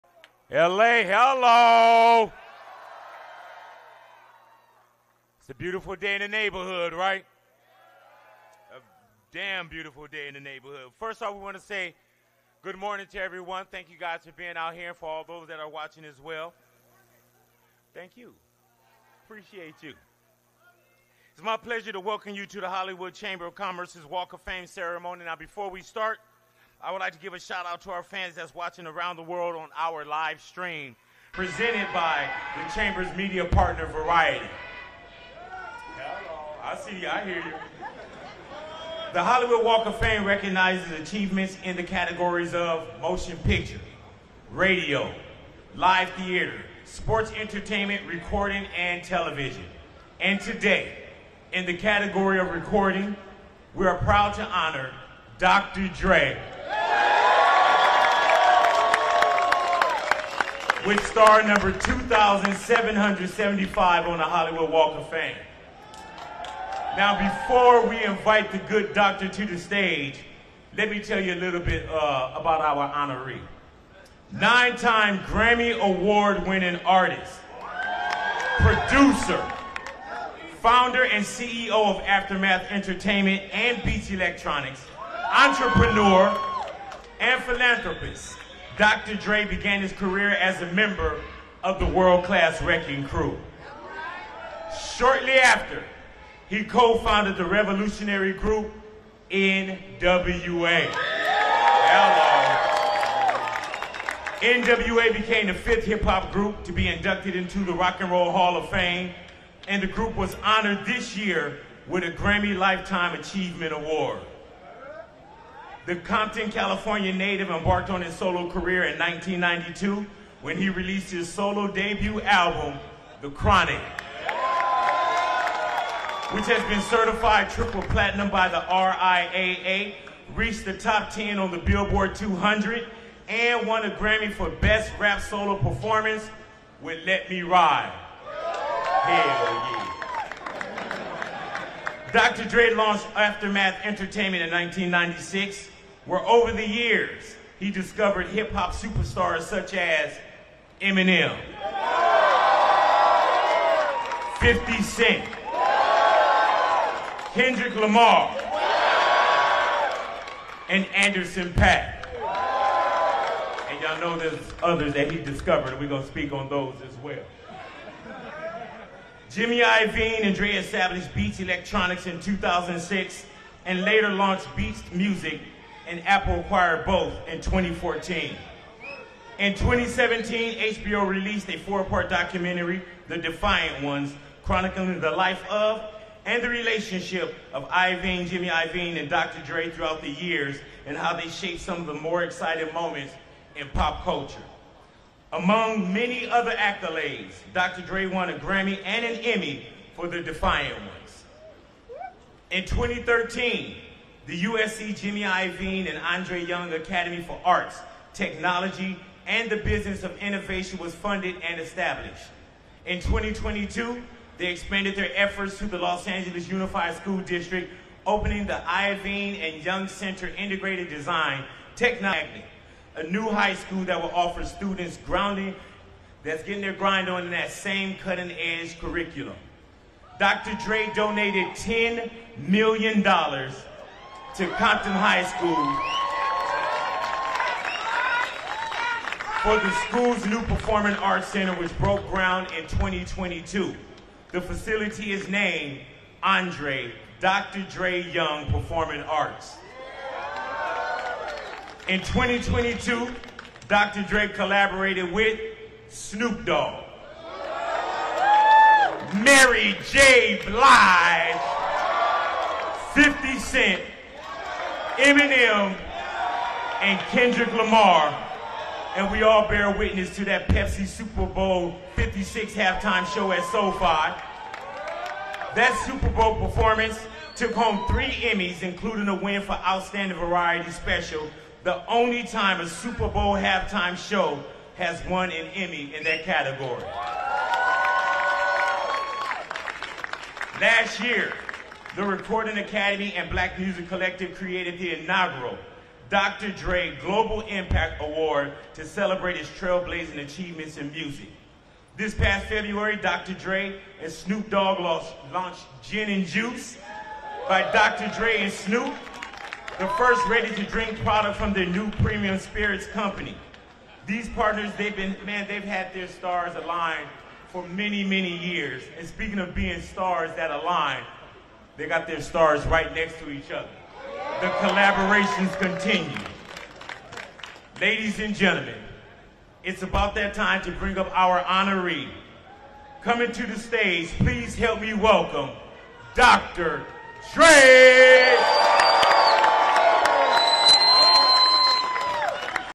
Watch: Eminem, 50 Cent, Snoop Dogg Reacts to BigBoy’s Speech About Dr. Dre | Walk of Fame 03.19.2024
Written by:
NK

Editorial Team:
NK
Kirill Bursov
Lilit Sagatelyan
Katerina Malik
Alexey Zakharenkov